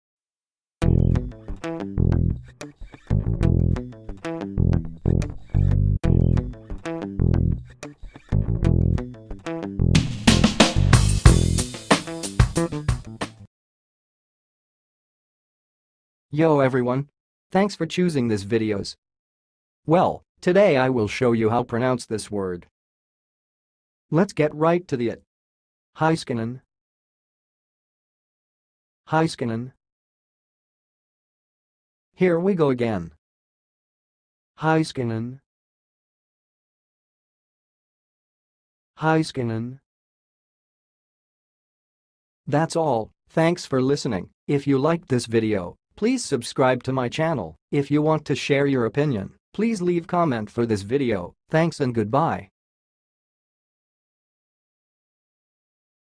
HEISKANEN - HOW TO PRONOUNCE IT!?
Watch how to say and pronounce "heiskanen"!